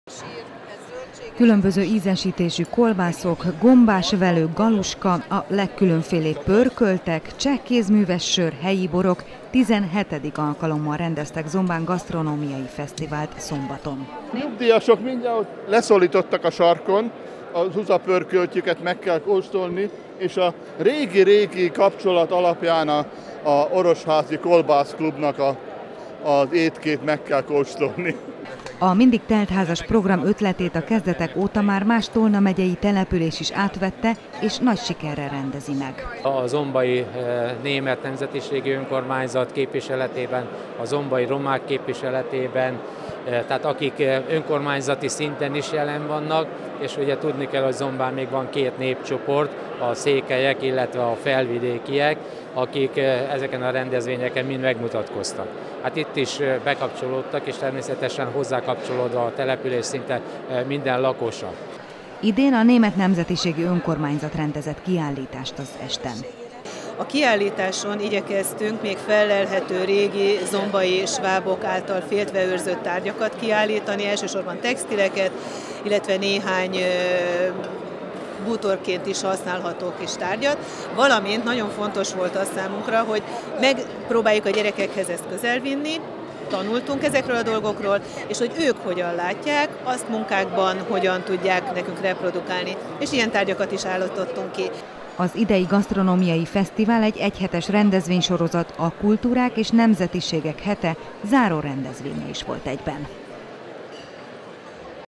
17. alkalommal rendezték meg Zombán az Ízek világa nevű gasztronómiai estet. 20181119
17. alkalommal rendezték meg Zombán a nemzetiségi hét záró akkordjaként az Ízek világa nevű gasztronómiai estet a település sportcsarnokában. Idén is a helyi nemzetiségek konyhájára helyeződött a hangsúly, így a közönség a német, roma, székely és felvidéki konyha ízeivel ismerkedhetett főként.